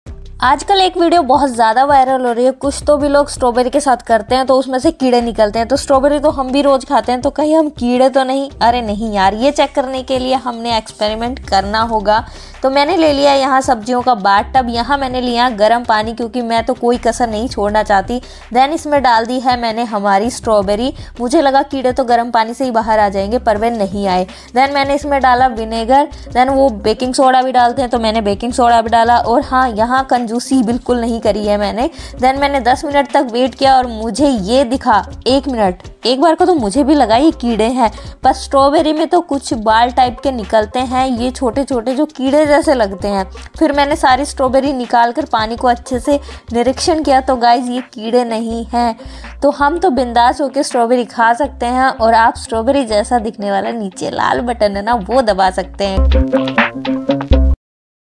Strawberry ke keede kaise nikale | US vlogs | Indians in US
आजकल एक वीडियो बहुत ज्यादा वायरल हो रही है कुछ तो भी लोग स्ट्रॉबेरी के साथ करते हैं तो उसमें से कीड़े निकलते हैं तो स्ट्रॉबेरी तो हम भी रोज खाता हैं तो कहानी हम कीड़े तो नहीं अरे नहीं यार यह चेक करने के लिए हमने एक्सपेरिमेंट करना होगा तो मैंने ले लिया यहां सब्जियां का बात तब यहां मैंने लिया ग्राम पानी क्योंकि मैं तो कोई कसार नहीं छोड़ना चाहती दें इसमें दाल दी है मैंने हमारी स्ट्रॉबेरी मुझे लगा किदर तो गम पानी से ही बाहर ए जाएंगे पर वेल नहीं आए दें मैंने इसमें डाला विनेगर दें वो बेकिंग सोडा भी डालते हैं तो मैंने बेकिंग सोडा अभी डाला और हां यहां कंजूसी बिल्कुल नहीं कारी है मैंने धन- मैंने 10 मिनट तक वेट किया और मुझे ये दिखा एक मिनट एक बार का तो मुझे भी लगा ही कीड़े है बस स्ट्रॉबेरी में तो कुछ बाल टाइप के निकलते हैं यह छोटे-छोटे जो कीड़े जैसे लगता हैं फिर मैंने साड़ी स्ट्रॉबेरी निकालकर पानी को अच्छे से निरीक्षण किया तो गैस ये कीड़े नहीं है तो हम तो बिंदास होकर स्ट्रॉबेरी का सकते हैं और आप स्ट्रॉबेरी जैसा दिखने वाला नीचे लाल बटन है ना वो ढाबा सकते हैं